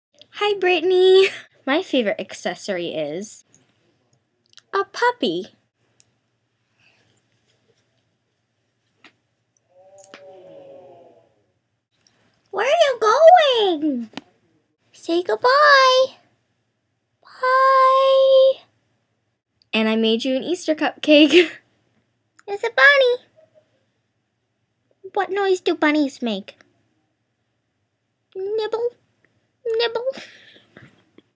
Response to SOS #64
I love my puppy Blue! He's a Cardigan Welsh Corgi :)